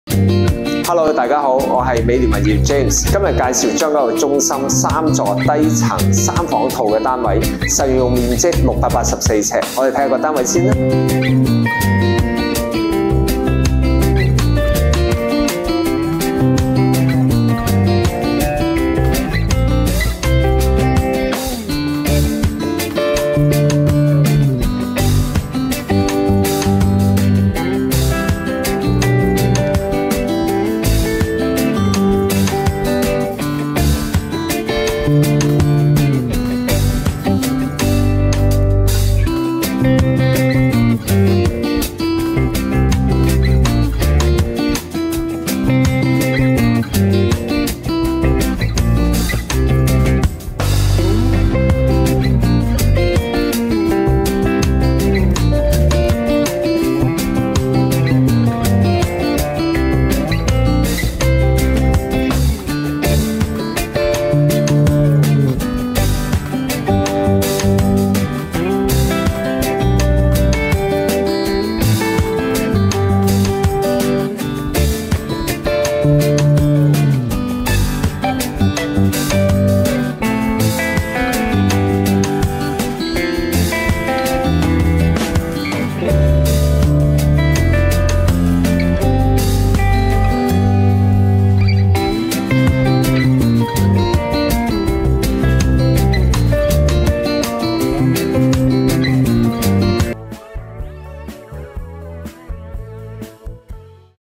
【代理James推介】將軍澳中心二期03座低層B室
KOL帶睇樓 – James Ho (S-162054)筍盤推介：

🏠將軍澳中心二期03座低層B室（物業編號：M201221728）

物業地址：寶琳 / 將軍澳站 唐德街9號
實用面積：684呎
間隔： 3房2廳
單位設施/特色：寵物天地
物業設施：基座商場
廣告日期：4/3/2022

屋苑：將軍澳中心 二期 全6座 2,280伙
入伙日期：22/01/2003
校網/校區：小學: 95; 中學: 西貢
發展商： 新鴻基 / 恒基兆業 / 南豐發展 / 華懋集團